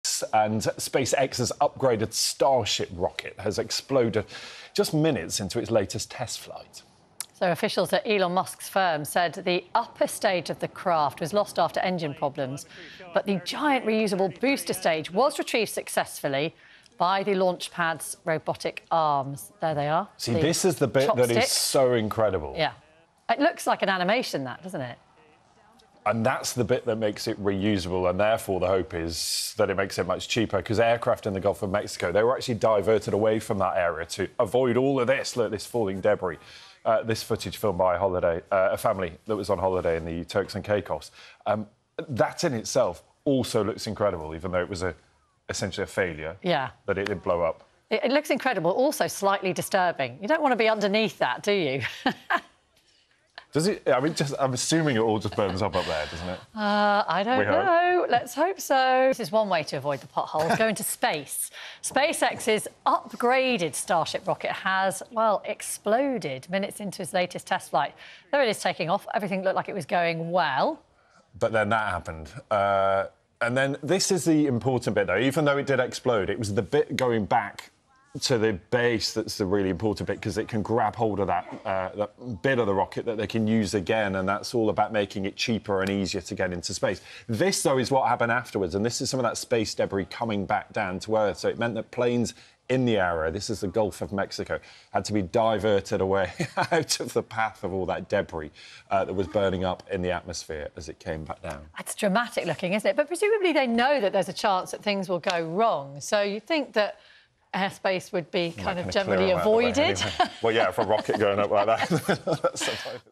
SpaceX launch half success & half failure in this test (USA) 17/Jan/2025
Booster returned a success - second stage blew up into a fiery light show.